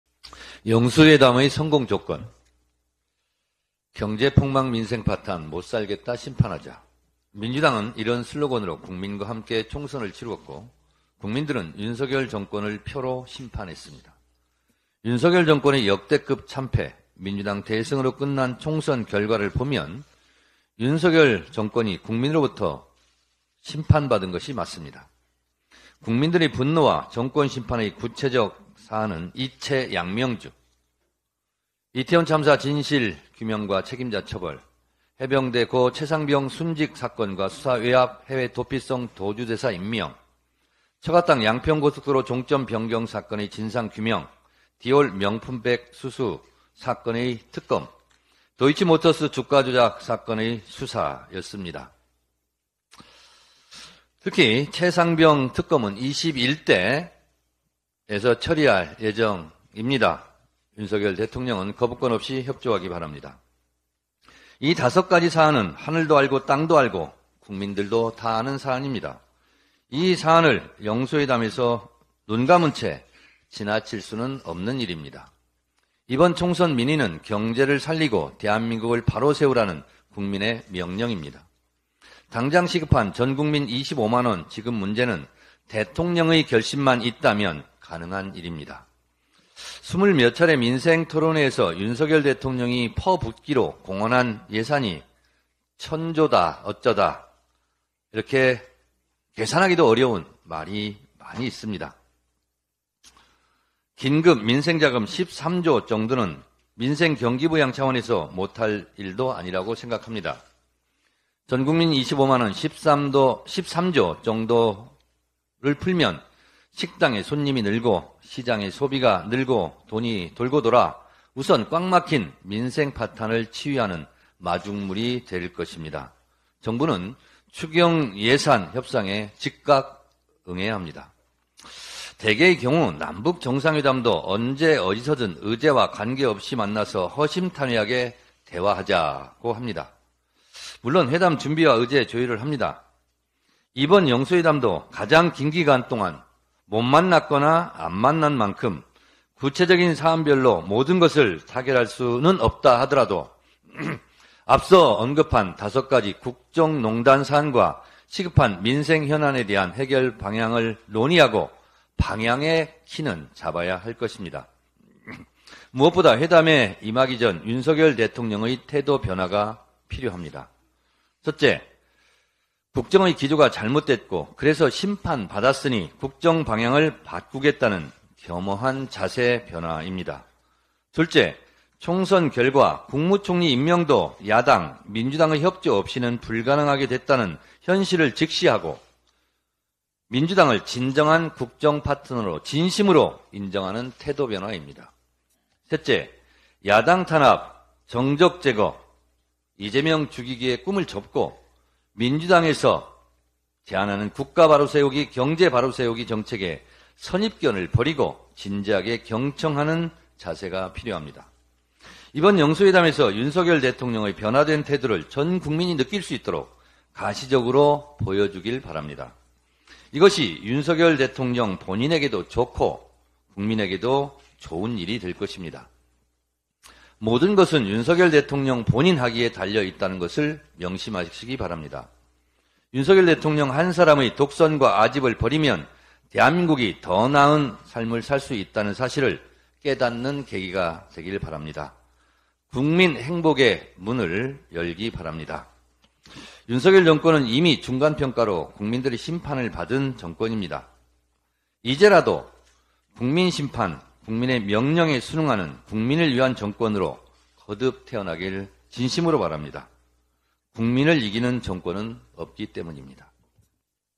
정청래가 제시한 영수회담 성공조건 “무엇보다 윤석열의 태도 변화 필요”
더불어민주당 최고위원회의가 22일 오전 국회 대표회의실에서 열렸다. 이 영상은 정청래 최고위원의 발언을 담고 있다.

( 영상 편집 : 강보현 PD, 윤다림 PD )